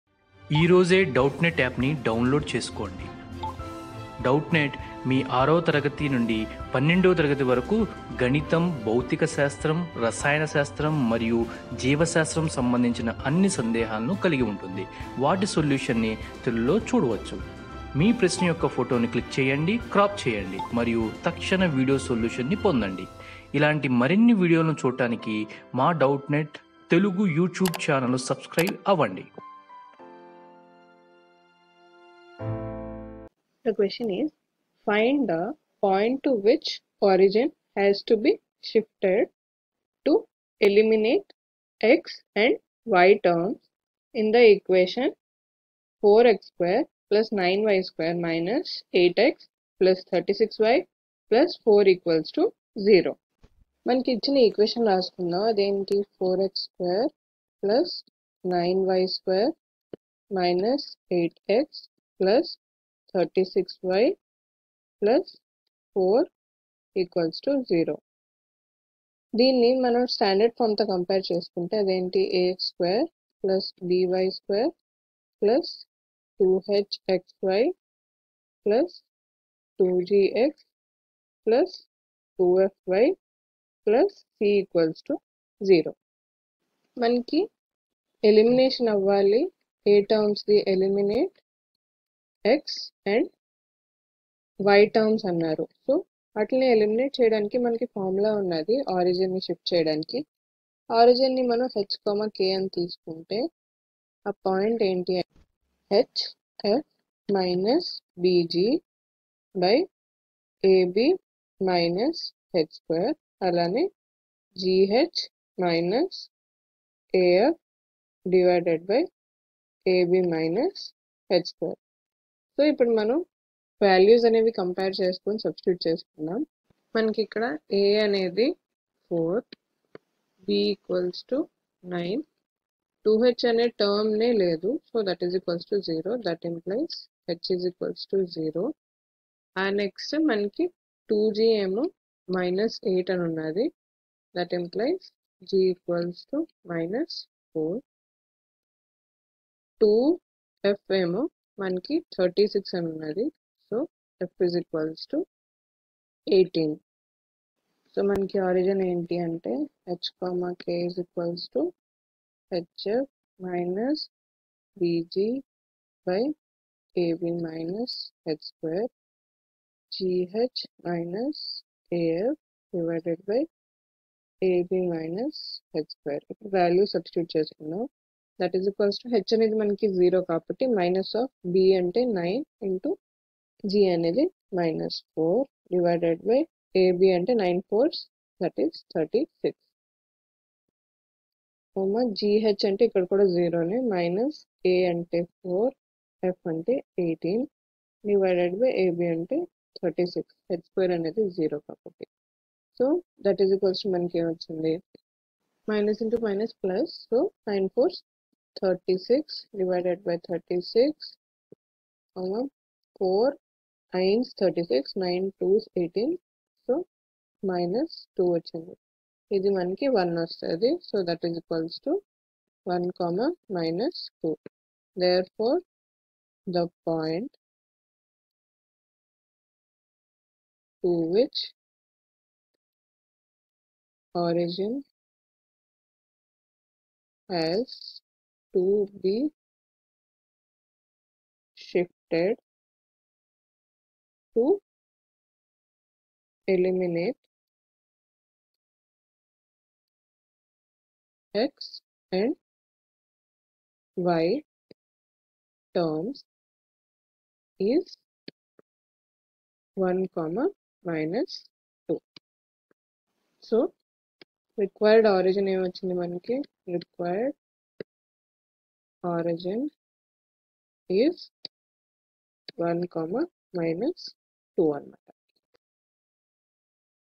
Find the point to which the origin is to be shifted so as to remove the |Class 12 MATH | Doubtnut
Find the point to which the origin is to be shifted so as to remove the first degree terms form the equation `4x^2+9y^2-8x+36y+4=0`.

Online Class में Admission लेने के लिए यहाँ click करें :
👉Class 12 | CBSE Boards | English Medium :

👉 Have Any Query? Ask Us.